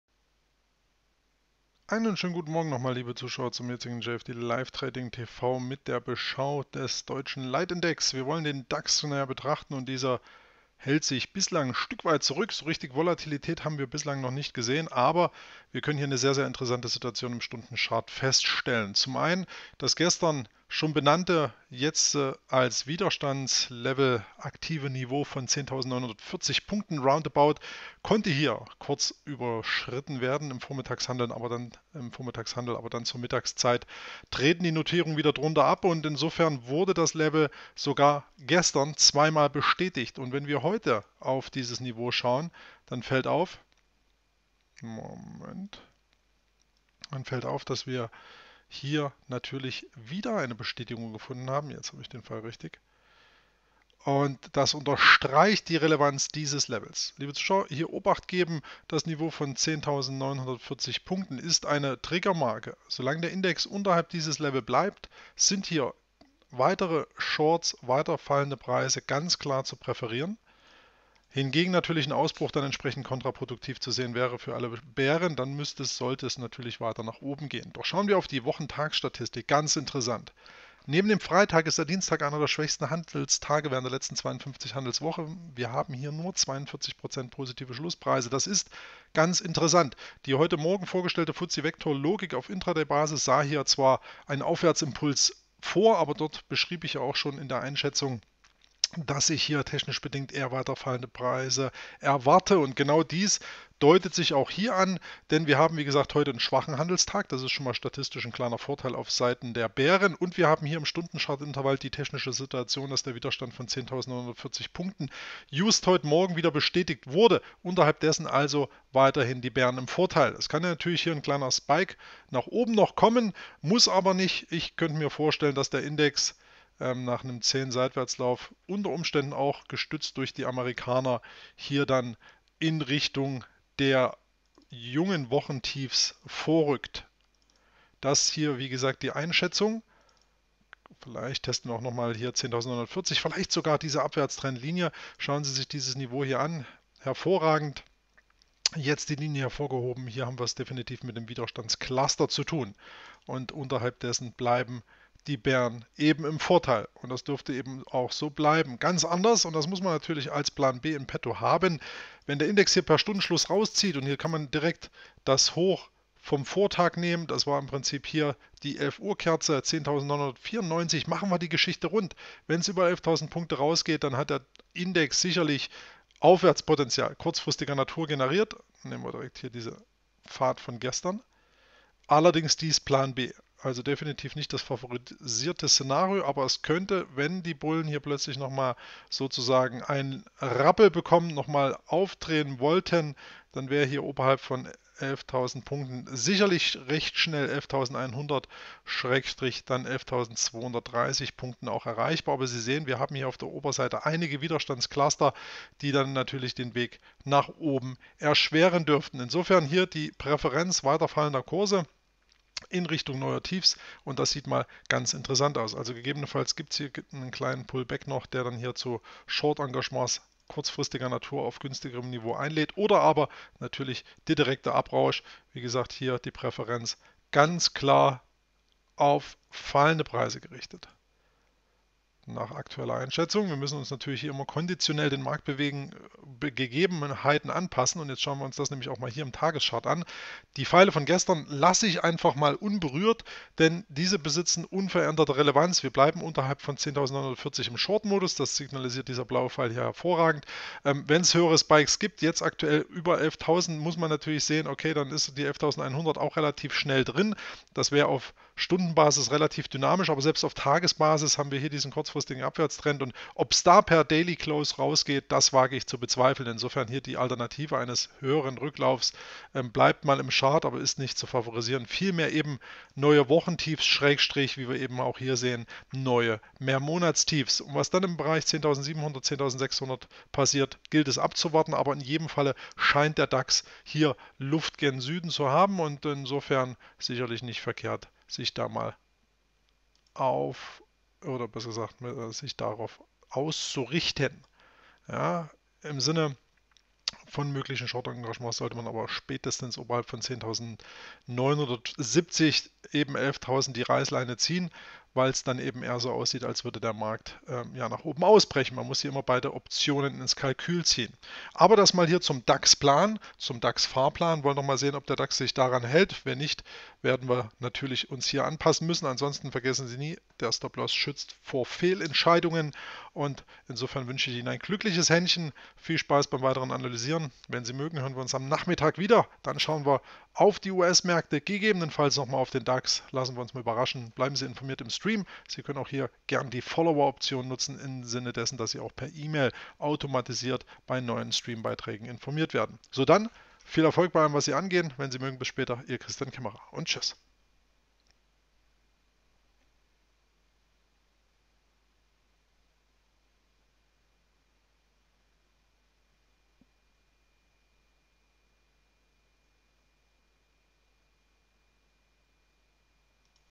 Der DAX bleibt schwach auf der Brust
Der DAX bleibt unterhalb des heute erneut bestätigten Widerstandslevels von 10.940 Punkten ein klarer Short. Neue Verlaufstiefs werden im Zusammenhang des schwachen Wochentages (42%) daher möglich.